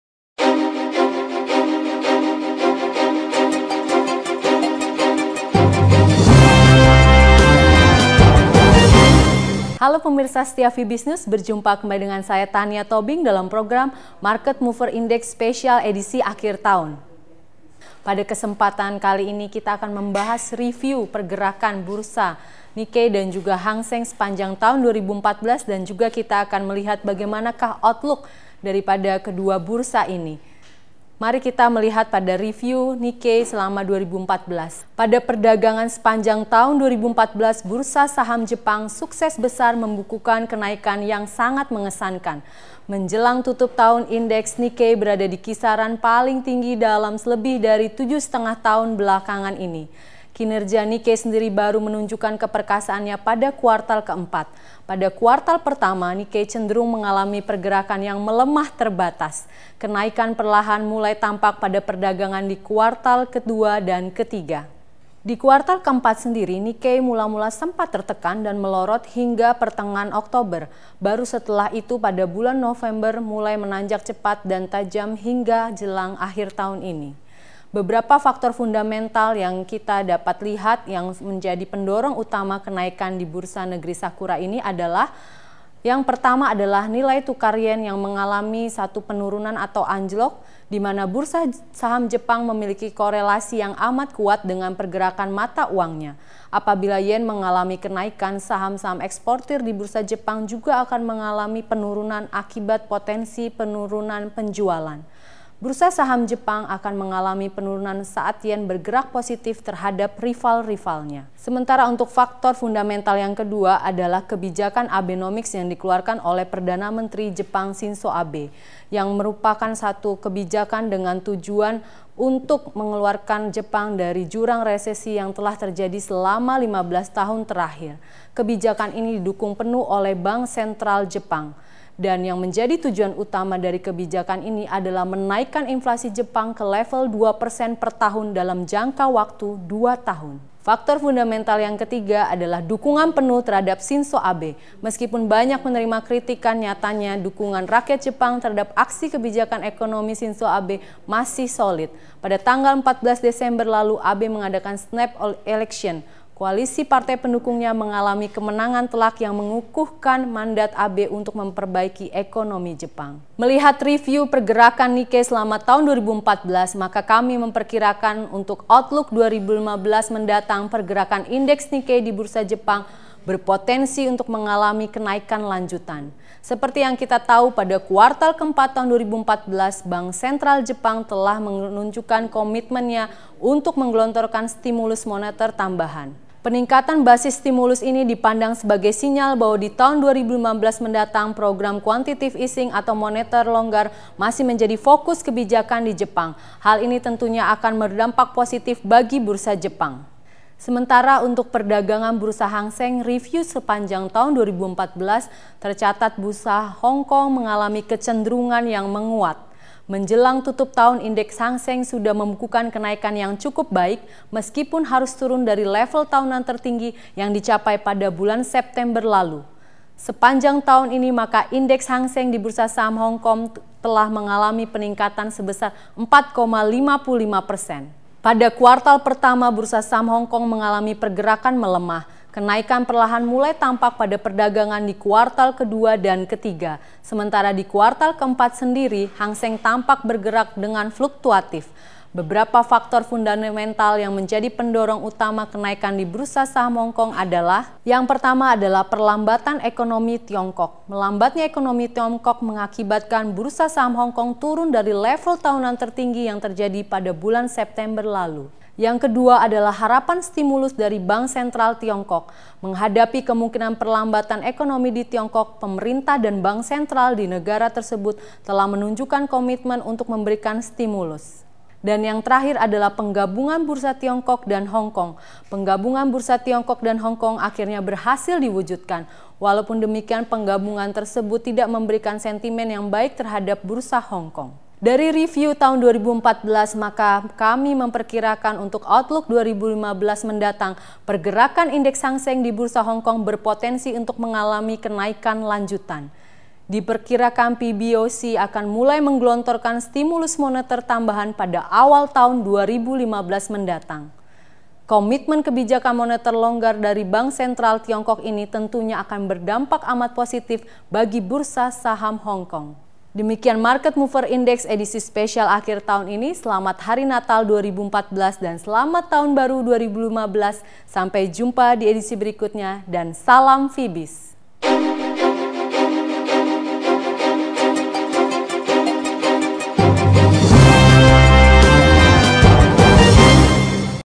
Review 2014, Outlook Nikkei dan Hangseng 2015 , Vibiznews 24 desember 2014
Program: Market Mover Index Spesial Outlook by Vibiznews
Edisi spesial ini, Tania Tobing mengulas pergerakan review indeks bursa saham Jepang dan juga bursa Hangseng. Pergerakan bursa saham Jepang menunjukkan suatu prestasi yang cukup mengesankan sehingga berdampak baik pergerakan tahun 2015. Demikian juga dengan kinerja bursa saham Hangseng yang menunjukkan prestasi di kuartal I dan II, dan dikuartal III bergerak flat namun masih dalam trend yang positif.